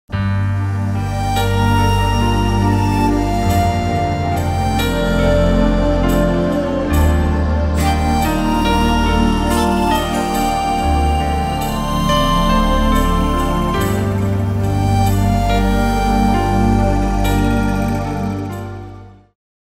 Storks Srinagar Migratory birds
Migratory storks at Srinagar, the summer capital of Jammu and Kashmir state, India.